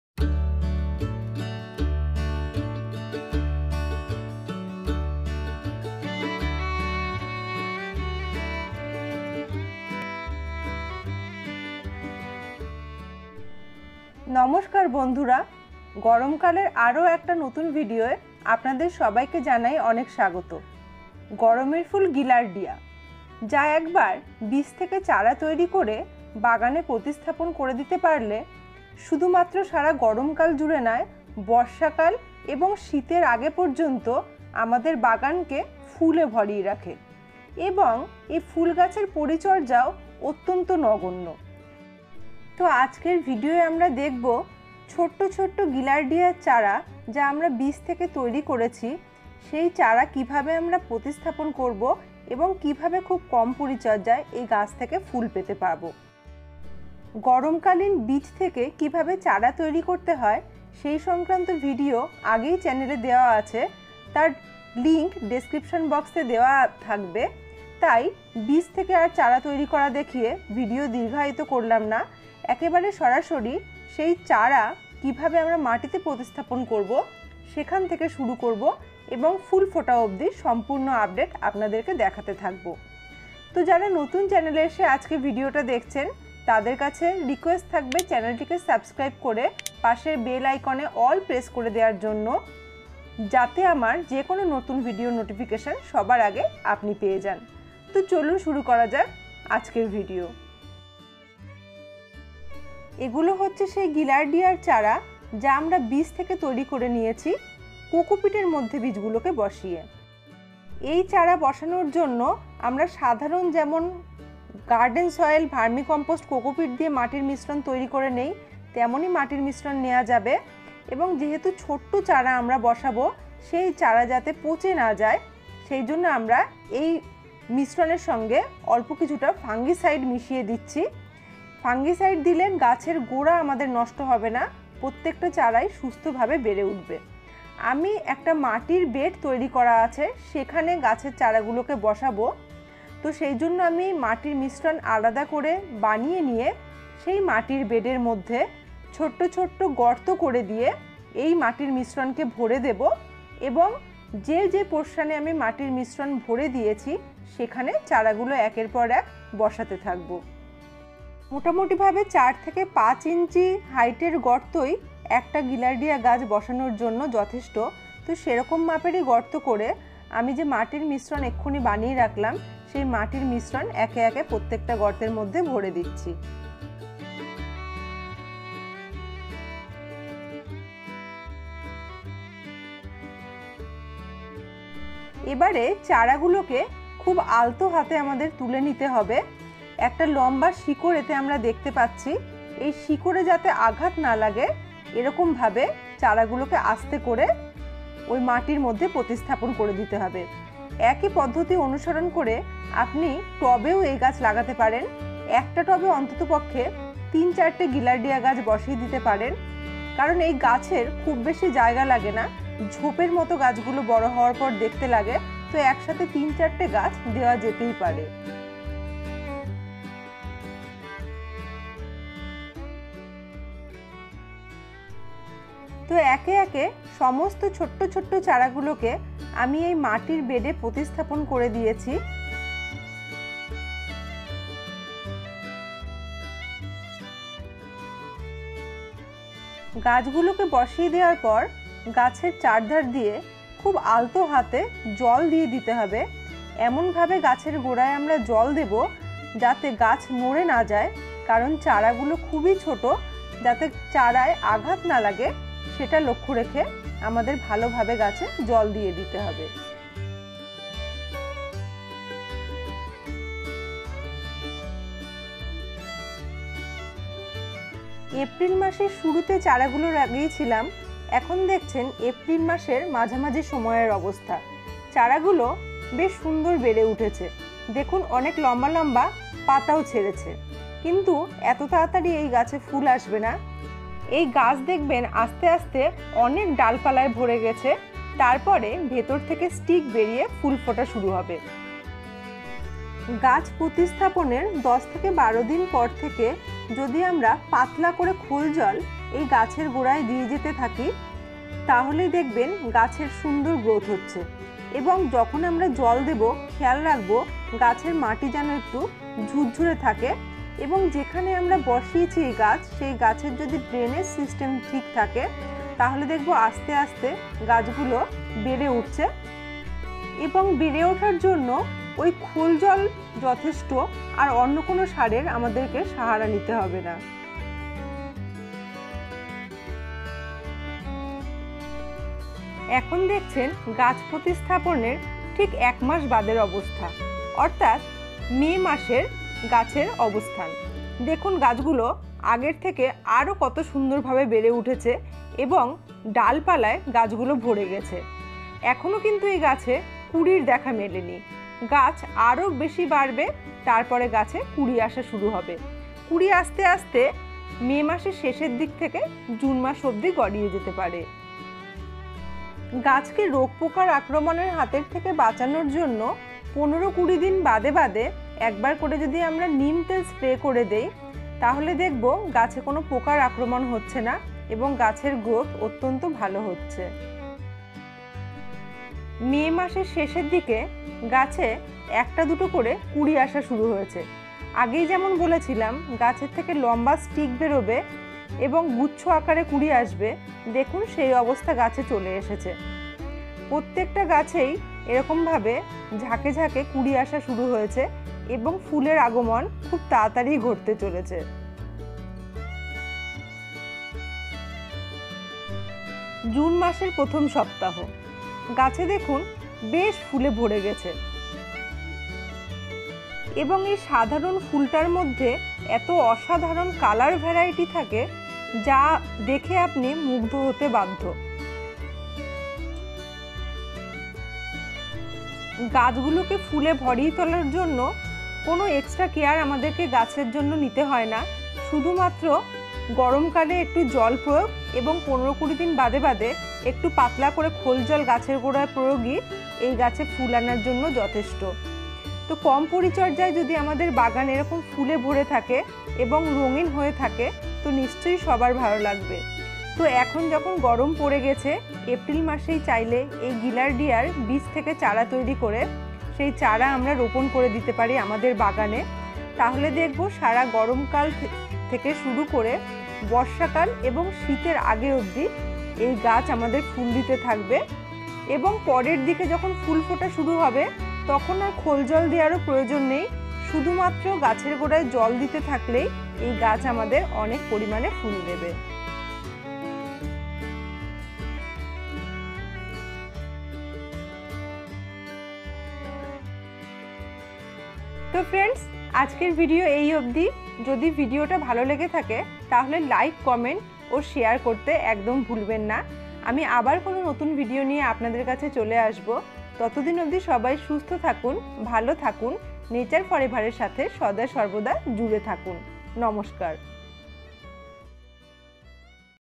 কম যত্নে বাগান ভরিয়ে রাখবে গিলার্ডিয়া ফুল//Grow Gaillardia flower very easily
এই ভিডিও তে গিলার্ডিয়া ফুল গাছের চারা থেকে ফুল ফোটা পর্যন্ত সম্পূর্ণ পরিচর্যা দেখানো হয়েছে।।

In this video the complete care of Gaillardia flower have been shown

করবেন। ধন্যবাদ 😊😊
Facebook page link for any query -

গাছের যে কোনো সমস্যার জন্য ফেসবুক পেজ এ যোগাযোগ করতে পারেন -